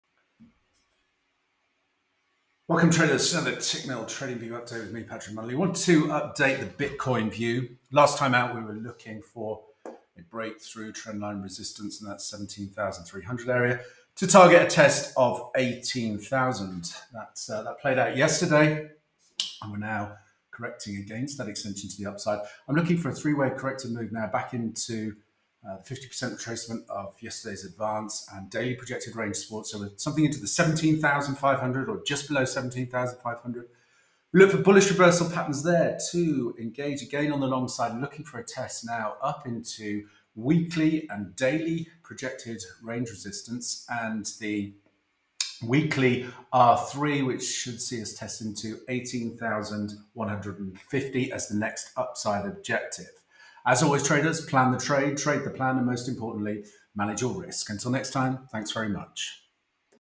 BTCUSD 18000 Target Hit, What Next?
In this update we review the recent price action in BTCUSD and identify the next high probability trading opportunity and price objectives to target